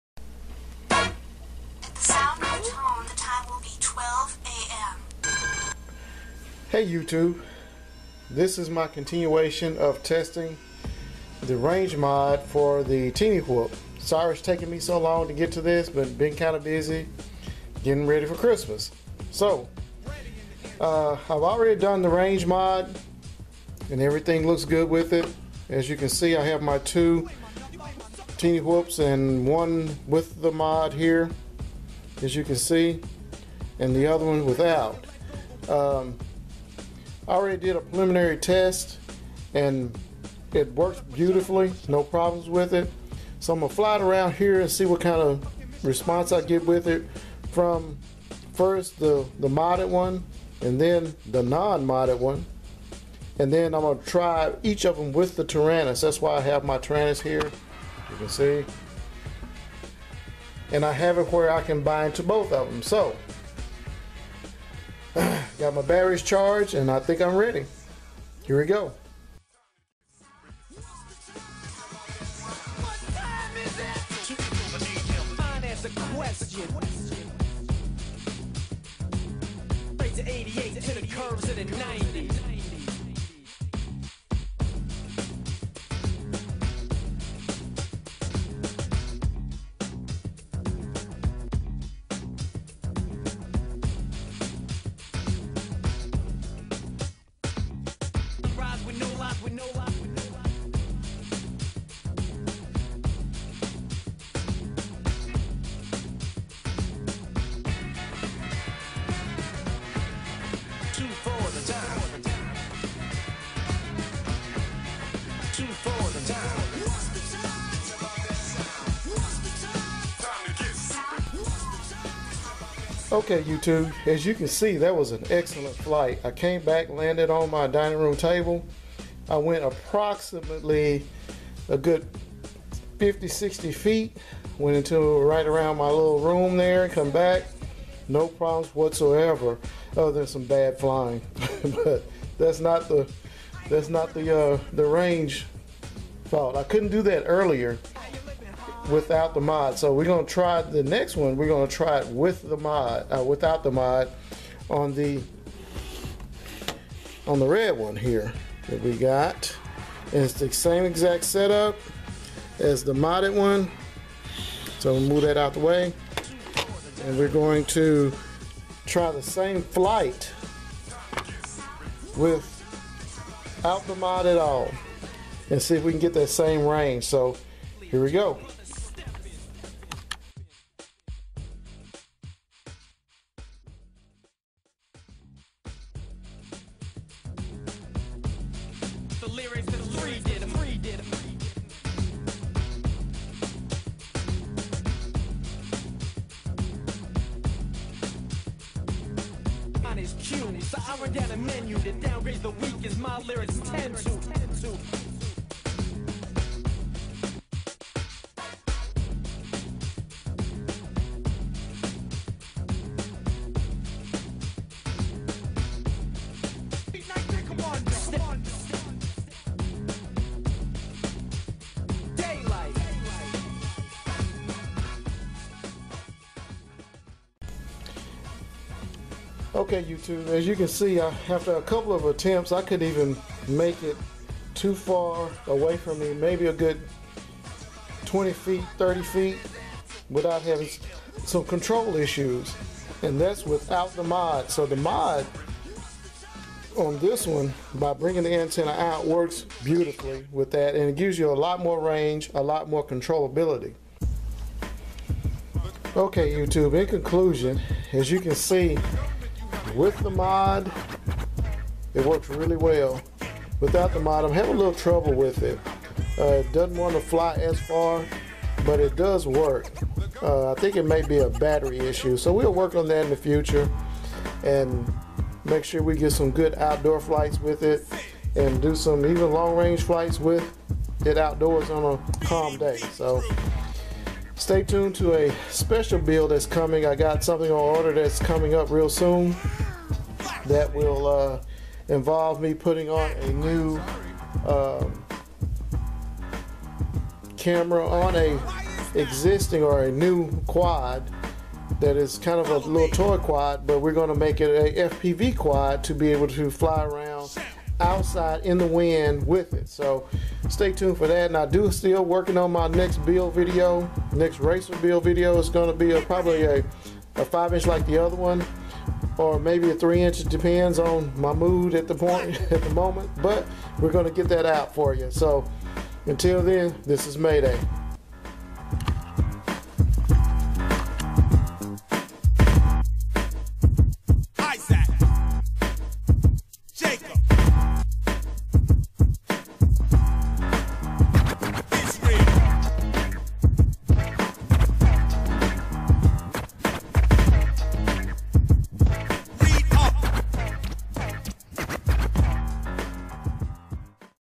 Tinywhoop range mod flight test
Here is the finial test for the range mod for the tiny whoop.